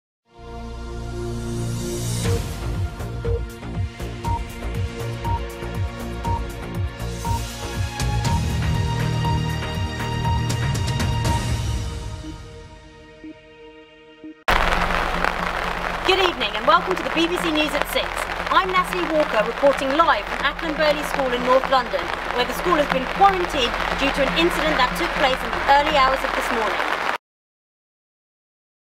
BBC News Report North London
It has been reported a serious incident has taken place at Acland Burghley school on the morning of 20th December. Nothing has been confirmed yet as the school would like maintain it's privacy, however, more details will be published in January.